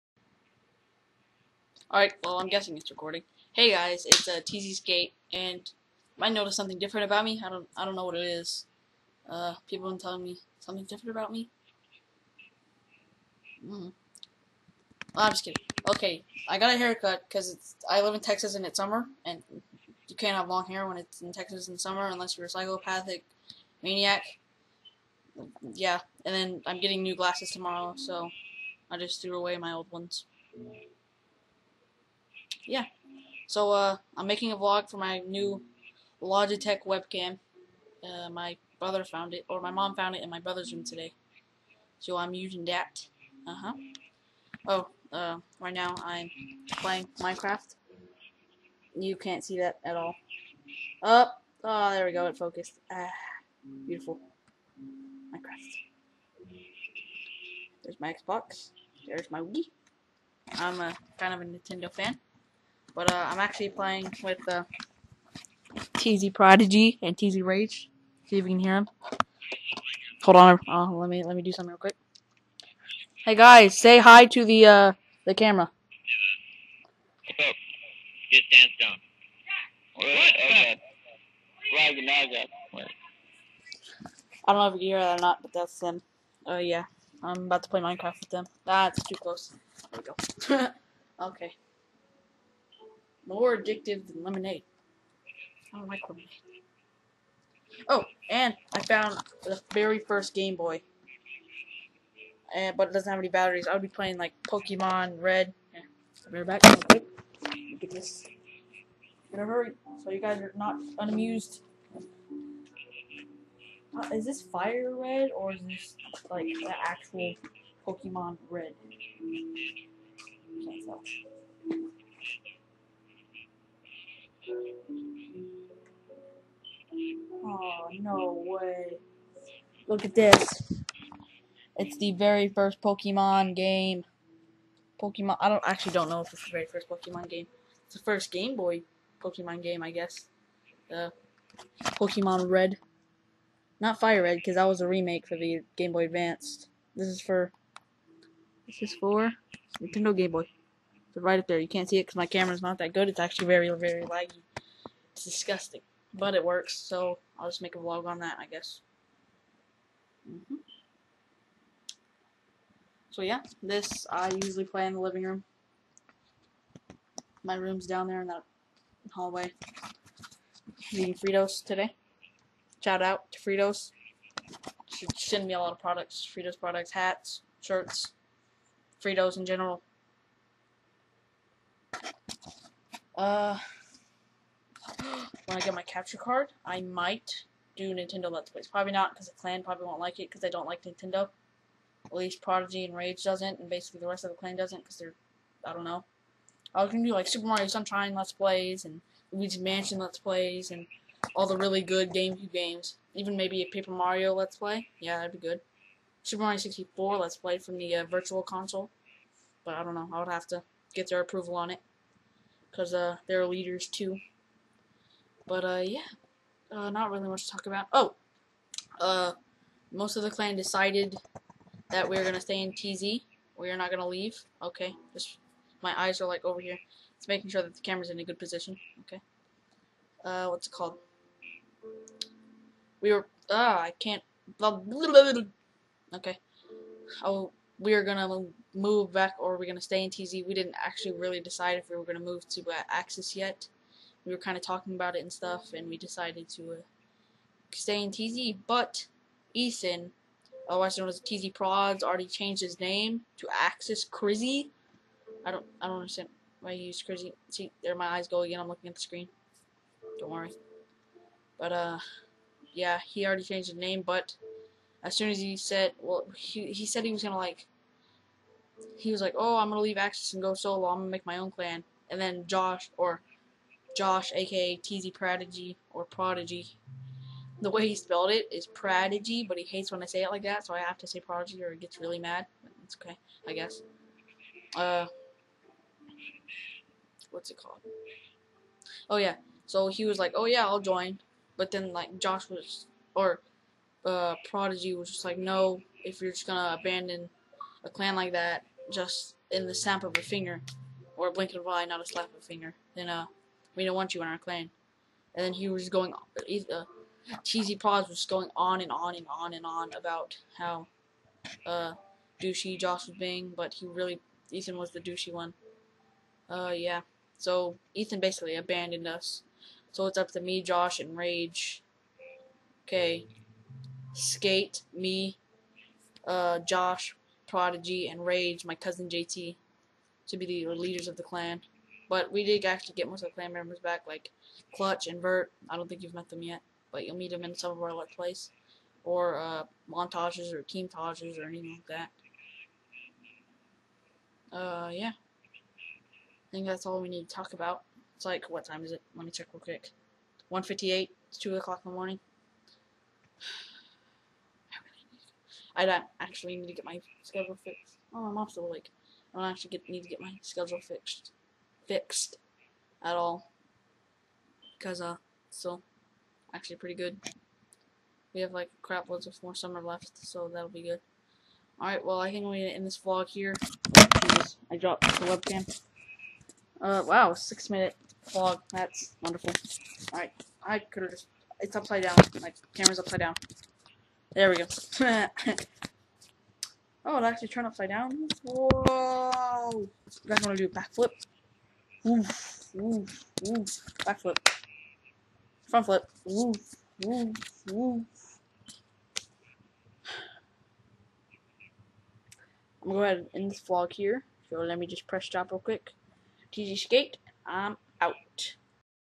Vlog With My New Webcam!!!!
Today i made a vlog with my new webcam! Does have some unfortunate news about the clan but it will get better